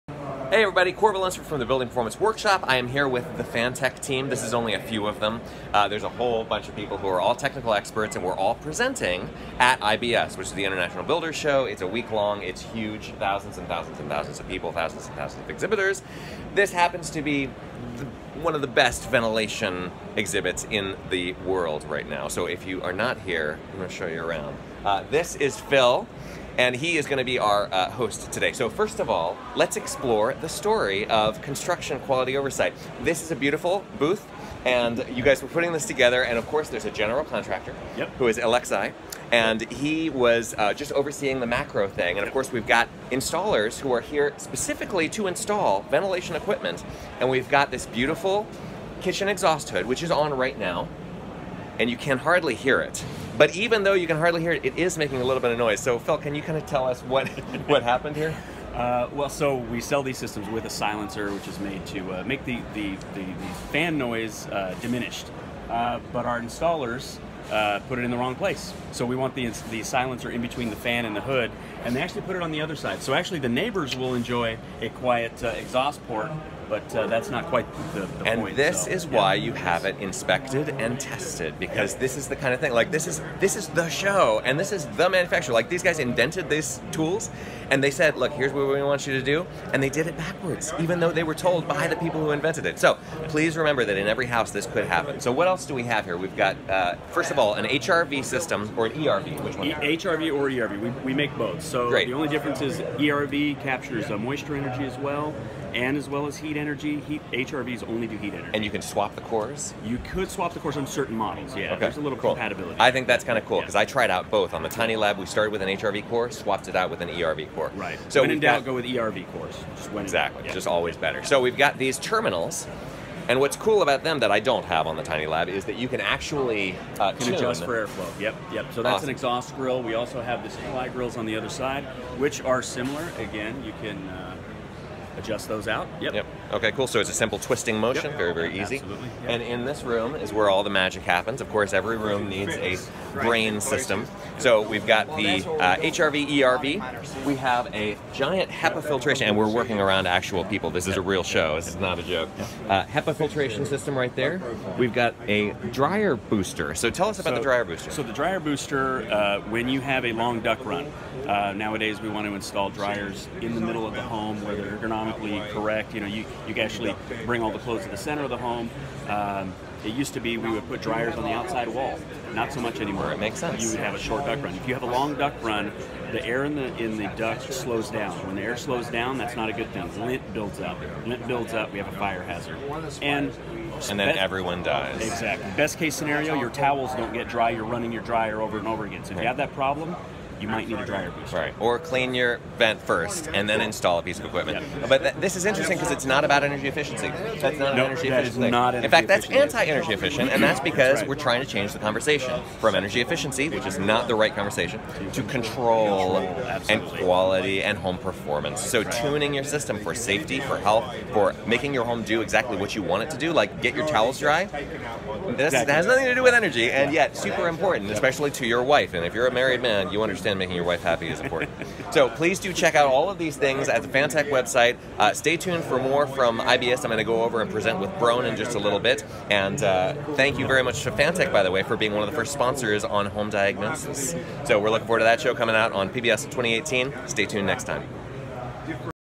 Fantech at IBS 2018: ERV/HRV, Inline Fan Silencers, Dryer Booster and HEPA Filtration
Phil Rivas of Fantech shows Corbett around the Fantech booth at the International Builders Show 2018. Demonstrations of Inline Kitchen Exhaust hood with silencer, Energy Recovery Ventilator with balancing registers, HEPA filtration, and Dryer Booster Fans.